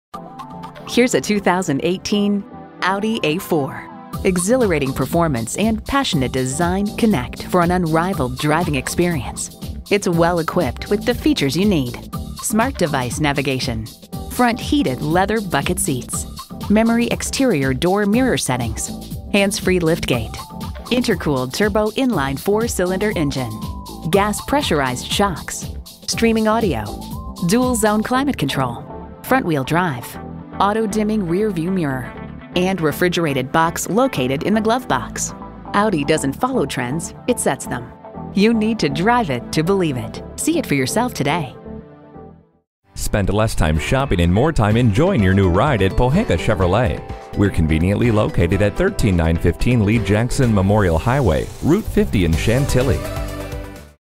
Used 2018 Audi A4 Manassas VA Chantilly, MD CP21054 - SOLD
Year: 2018
Make: Audi
Model: A4
Trim: 2.0T
Engine: 2.0L I4 TFSI DOHC
Transmission: 7-Speed Automatic S tronic
Color: Silver
Interior: Nougat Brown/Black
Mileage: 35181
Stock #: CP21054
VIN: WAULMAF49JN007516

Address:
13915 Lee Jackson Hwy